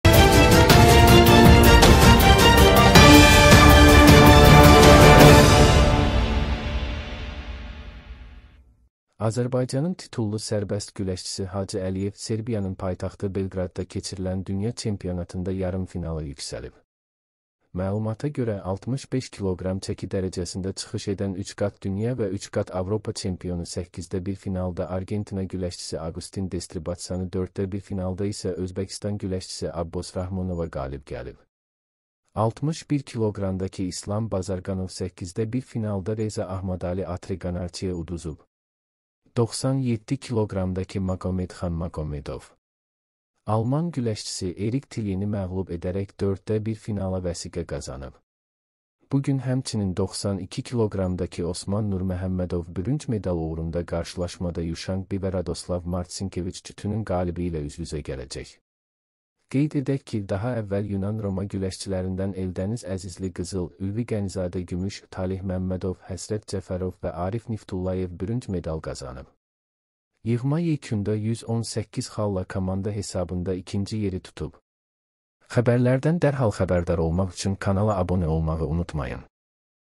Hacı Əliyev dünya çempionatında yarımfinala yüksəlib
Azərbaycanın titullu sərbəst güləşçisi Hacı Əliyev Serbiyanın paytaxtı Belqradda keçirilən dünya çempionatında yarımfinala yüksəlib.

Məlumata görə, 65 kq çəki dərəcəsində çıxış edən 3 qat dünya və 3 qat Avropa çempionu 1/8 finalda Aqustin Destribatsa (Argentina), 1/4 finalda isə Abbos Rahmonova (Özbəkistan) qalib gəlib.

61 kq-dakı İslam Bazarqanov 1/8 finalda Reza Ahmadali Atreqanarçiyə (İran) uduzub. 97 kq-dakı Maqomedxan Maqomedov Erik Tilyeni (Almaniya) məğlub edərək 1/4 finala vəsiqə qazanıb.

Bu gün həmçinin 92 kq-dakı Osman Nurməhəmmədov bürünc medal uğrunda qarşılaşmada Yuşanq Bi (Çin) / Radoslav Martsinkeviç (Polşa) cütünün qalibi ilə üz-üzə gələcək.

Qeyd edək ki, daha əvvəl yunan-Roma güləşçilərindən Eldəniz Əzizli (55 kq) qızıl, Ülvi Qənizadə (72 kq) gümüş, Taleh Məmmədov (63 kq), Həsrət Cəfərov (67 kq) və Arif Niftullayev (97 kq) bürünc medal qazanıb. Yığma yekunda 118 xalla komanda hesabında 2-ci yeri tutub.

Mənbə report.az